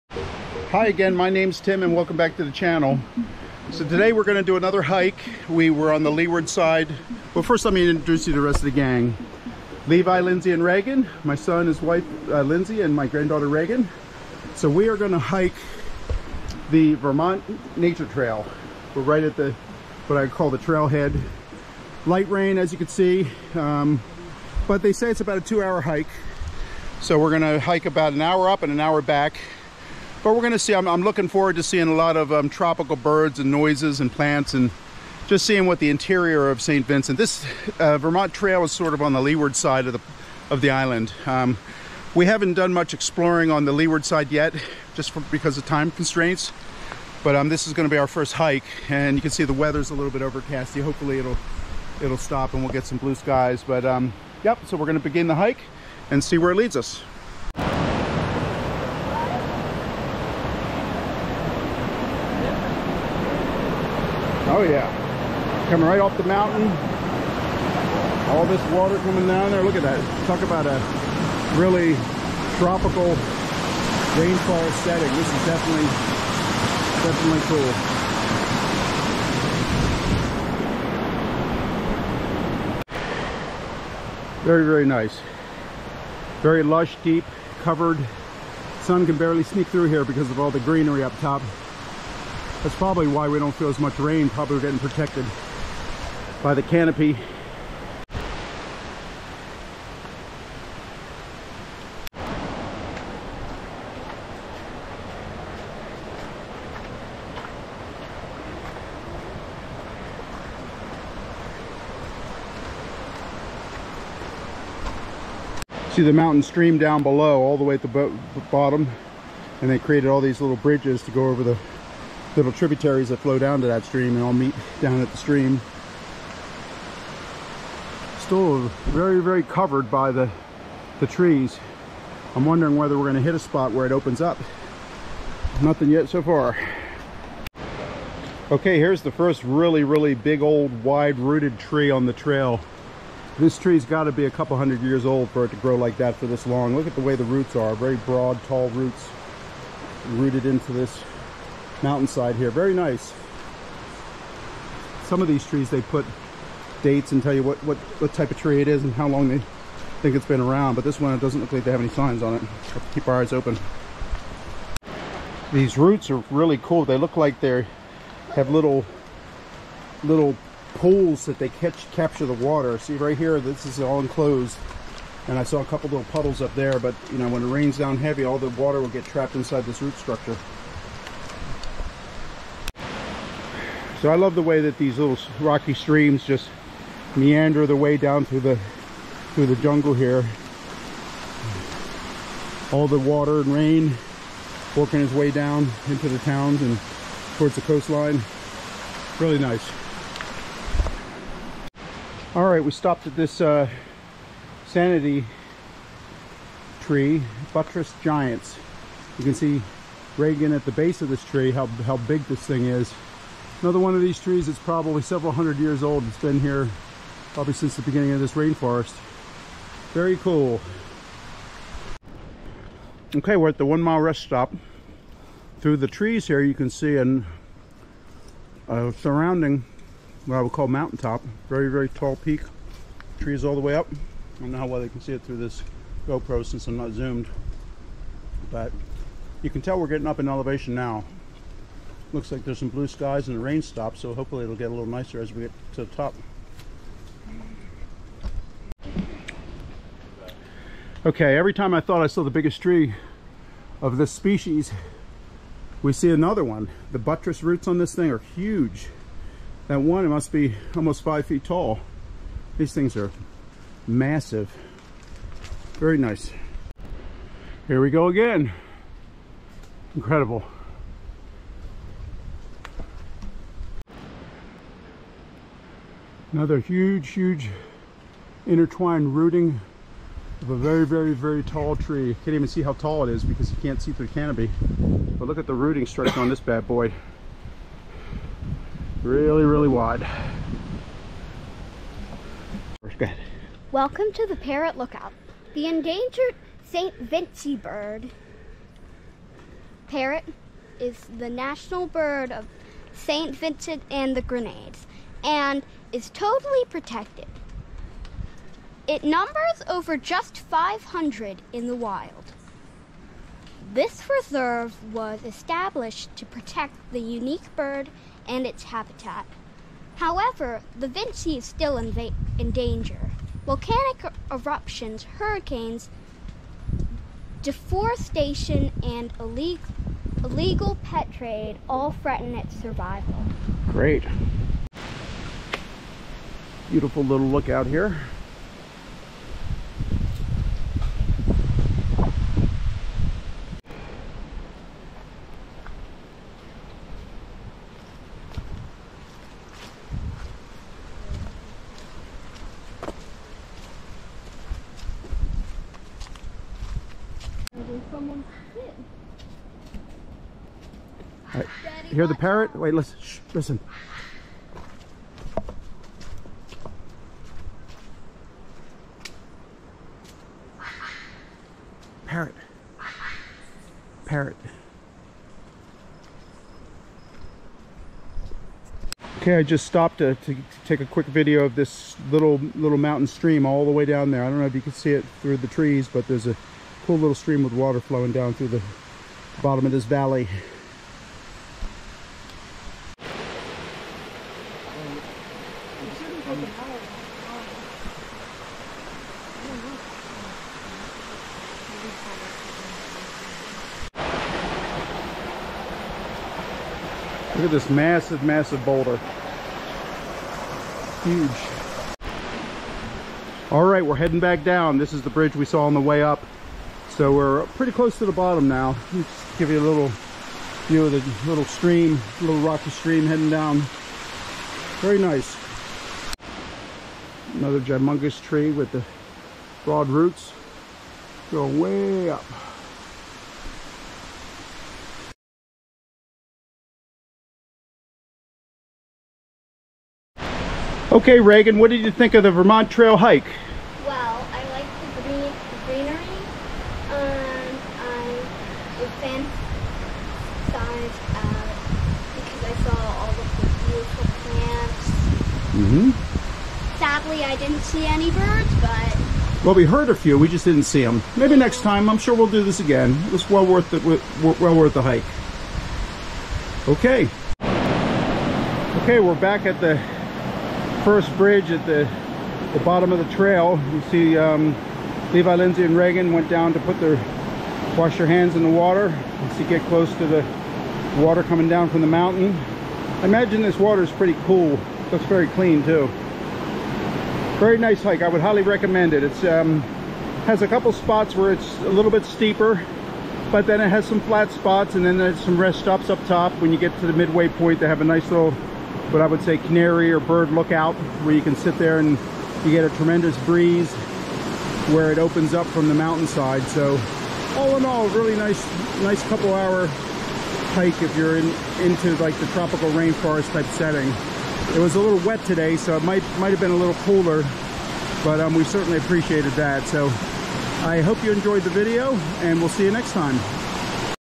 Vermont Nature Trail Hike, St. Vincent and the Grenadines
The Vermont Nature Trail takes you through lush rainforest with an amazing variety of tropical flora, ending up with beautiful scenery and a chance to see the famous St. Vincent Parrot in its natural habitat. Majestic cliffs reach for the sky, giving way to sweeping valleys. Ambient temperatures and abundant rainfall give rise to profusion in the growth of vegetation. The trail is in the picturesque Buccament valley within the St. Vincent Parrot Reserve and the more encompassing proposed Central Forest Reserve.
 The trail is approximately 2 miles (3.5 km) long, and winds through lush primary and secondary rainforest and plantations, offering visitors breath-taking panoramic views of the surrounding landscape and close-ups of the country’s endemic flora and fauna. The trail ascends to the lookout and offers great hiking for all ages and abilities.